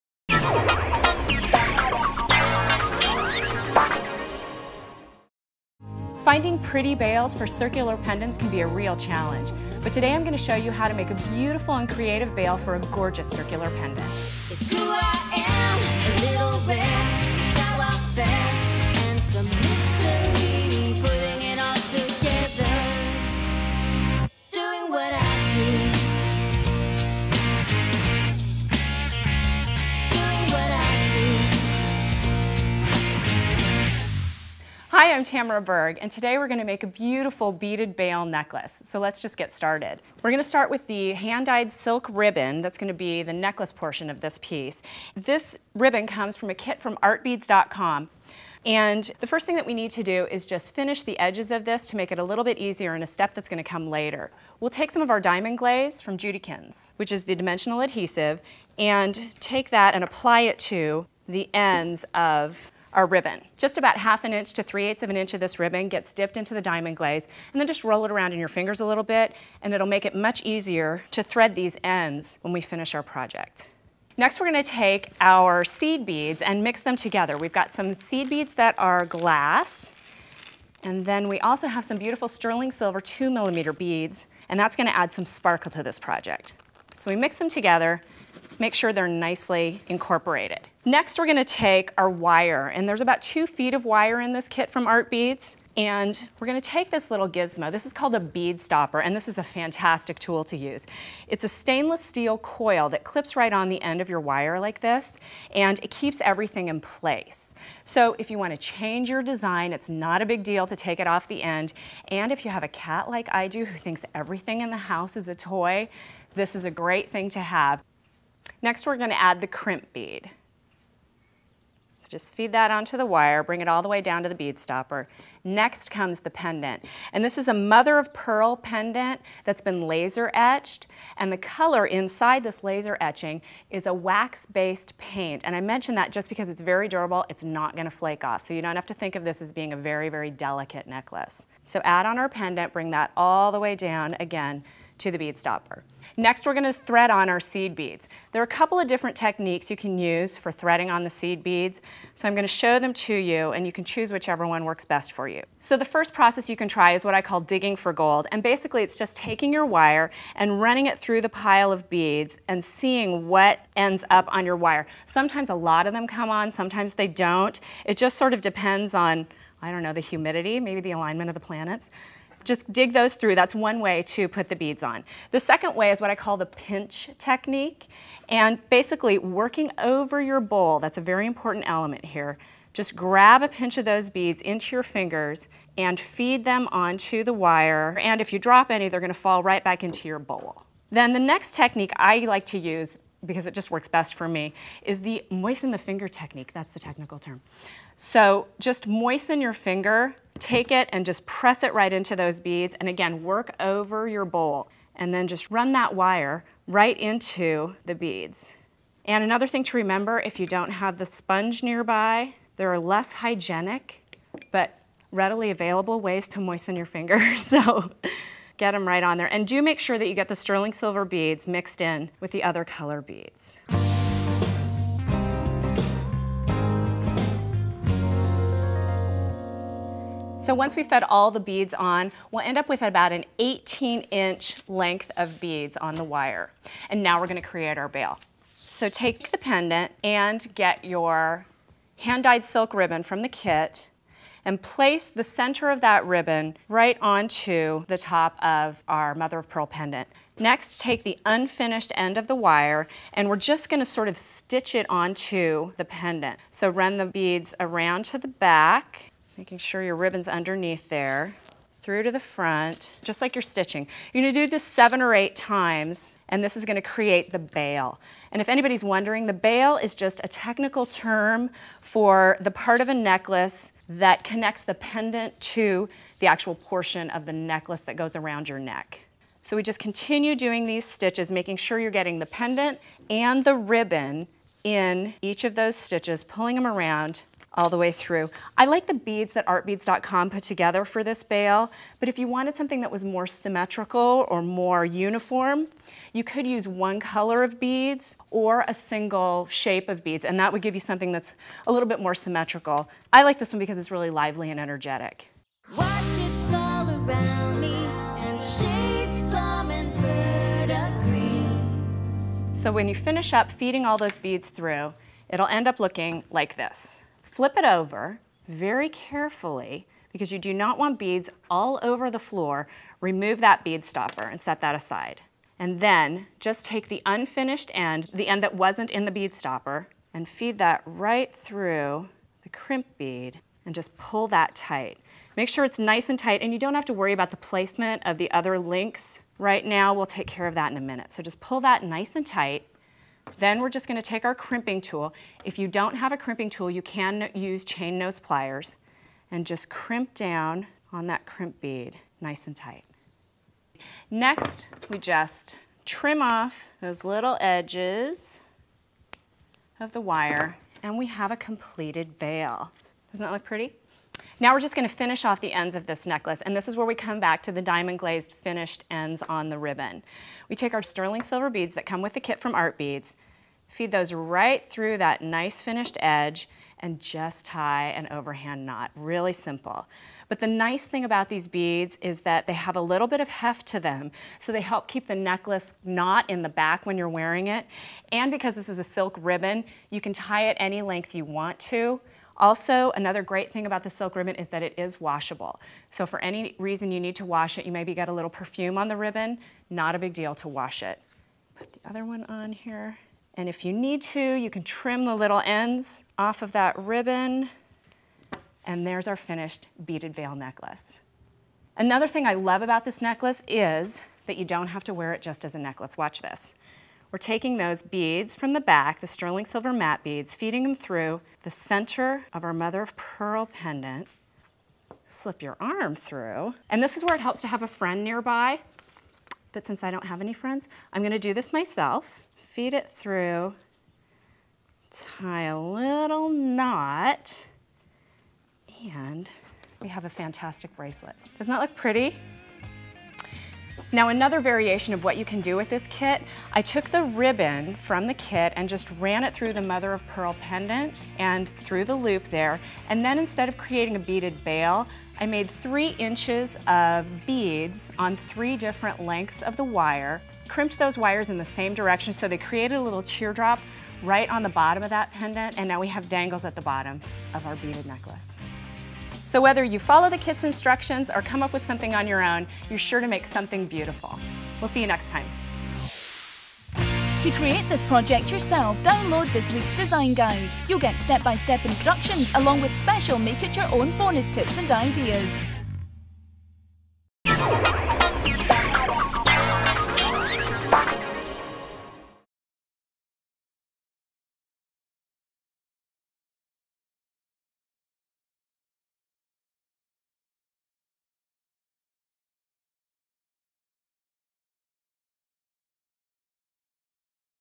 Beaded Bail Necklace
Tamara Berg joins us with an elegant beaded necklace. Inspired by the secrets of the sea, a mother-of-pearl focal piece is just the beginning for this jewelry artist. With a flick of the wrist this necklace becomes a unique bracelet.